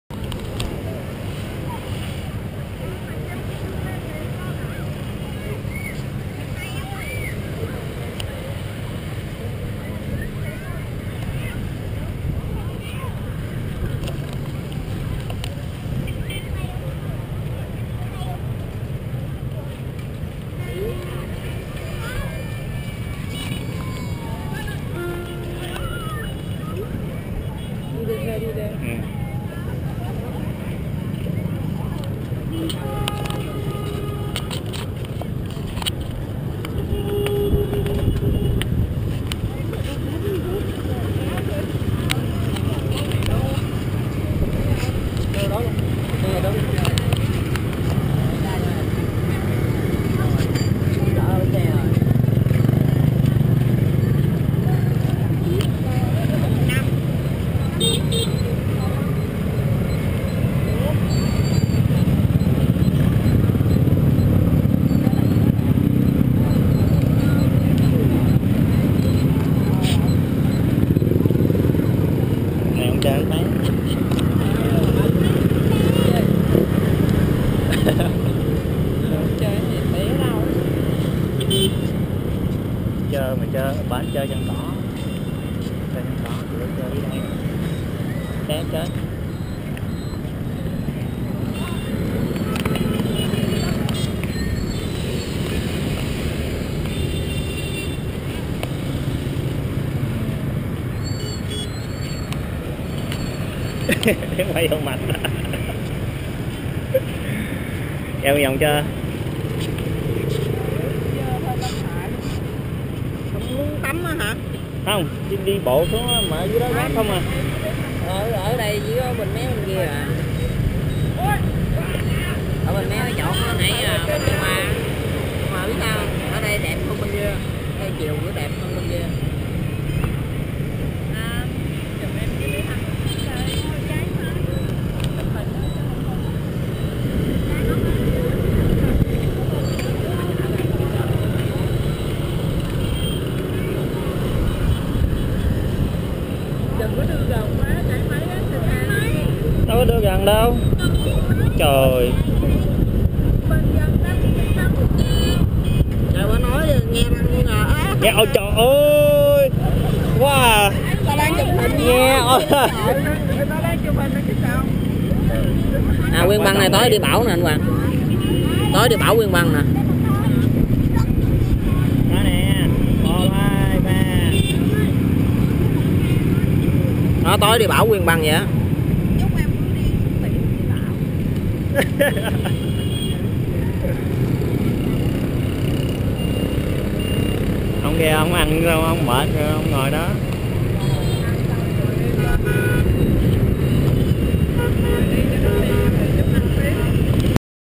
A walk on Vung Tau beach
Me and Kailey went for a walk